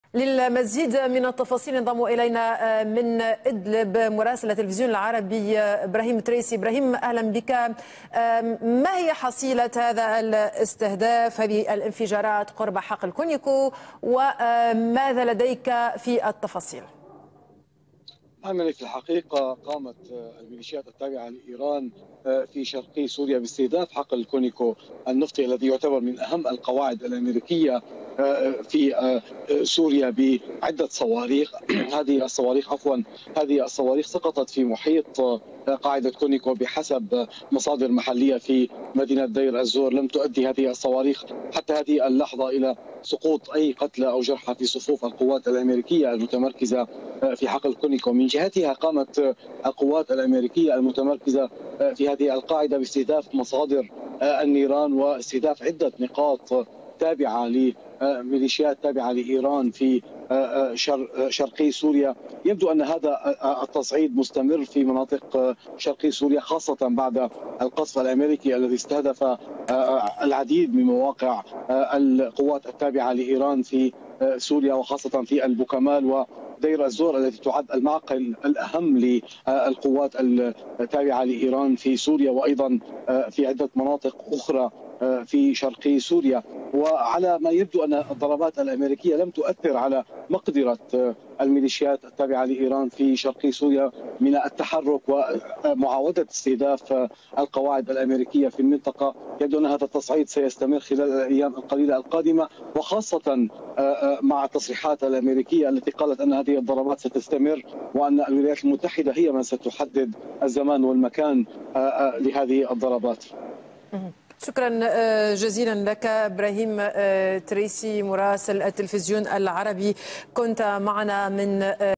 مراسل العربي إبراهيم تريسي: تصعيد وانفجارات بحقل كونيكو النفطي بريف دير الزور شرقي سوريا
قال مراسل العربي إبراهيم تريسي من إدلب إن استهدافات لميليشيات قريبة من إيران قرب حقل كونيكو النفطي بريف دير الزور شرقي إيران الذي يعتبر أهم القواعد الأميرية عبر صواريخ سقطت قرب الحقل ولم تؤدي إلى قتلى أو جرحى، وأضاف بأن الرد الأميركي استدهدف مصادر النيران في تصعيد مستمر  بعد ضرب مواقع قوات تابعة لإيران في سوريا، وأضاف بان الضرابات الأاميركية لم تضعف قدرات هذه المجموعات.